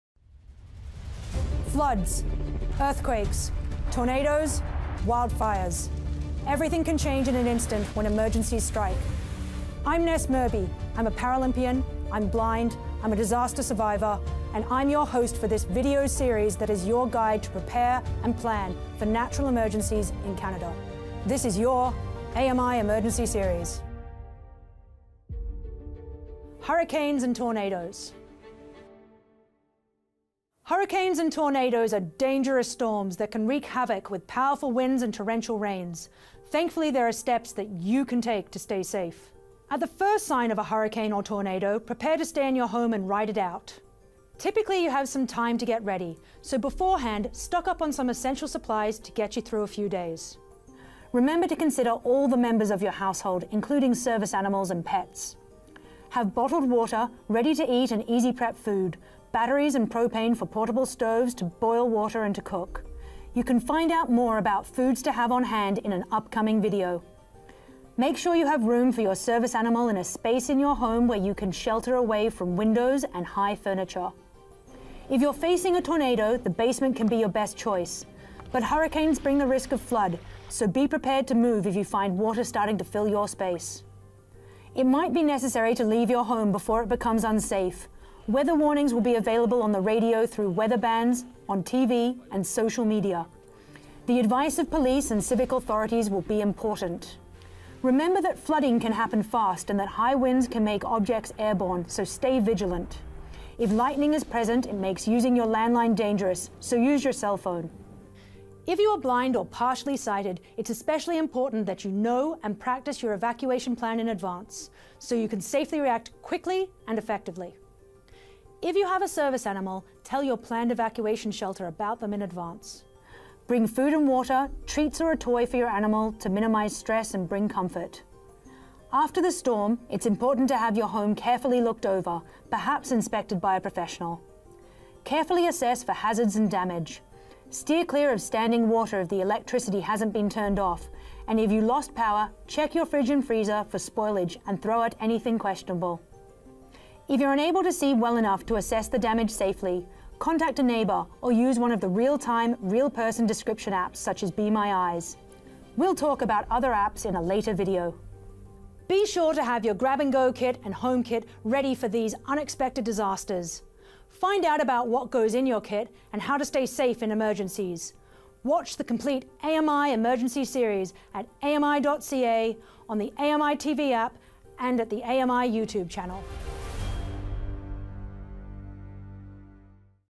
Hurricanes and Tornadoes
For those in tornado or hurricane-prone areas, the threat of these storms can’t be overstated. Vicious fast winds, debris, and rain or hail can put those in the path of a hurricane or tornado in grave danger. Ness Murby breaks down the steps before, during, and after a hurricane or tornado.